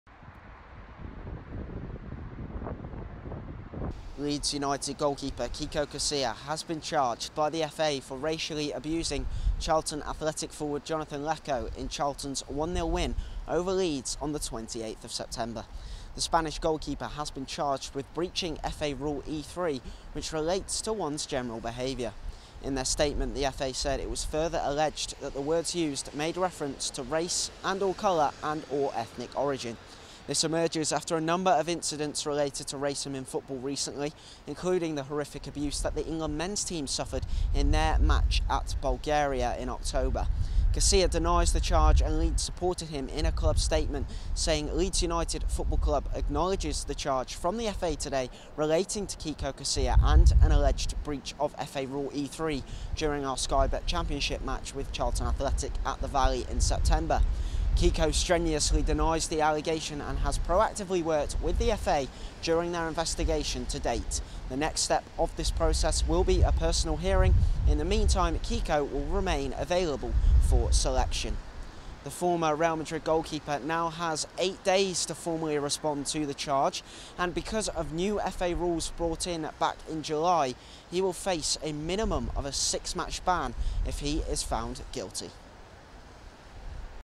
Kiko Casilla Denies FA Charge
🗞 Kiko Casilla denies the FA charge of racially abusing Charlton striker Jonathon Leko